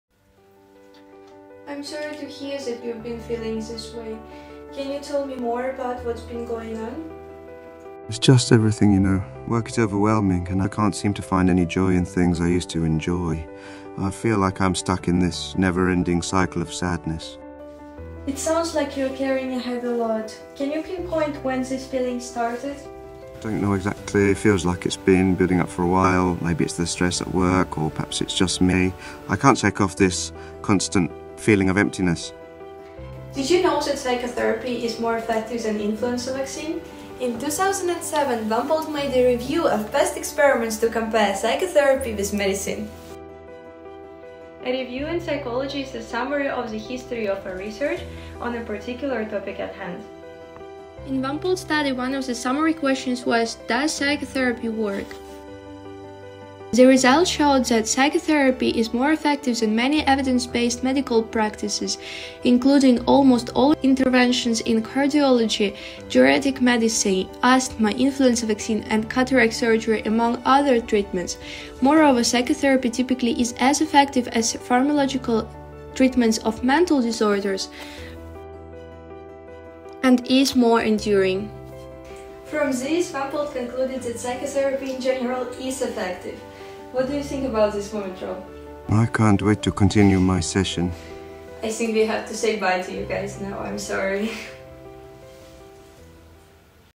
7. Psychotherapy is More Effective than Influenza Vaccine
I’m sorry for a delay, it took me some time to create Moomin’s speech with AI. Do you want more videos with Moomintroll?

P.s. the painting is “The Luncheon on the Grass” by the French artist Édouard Manet (1863)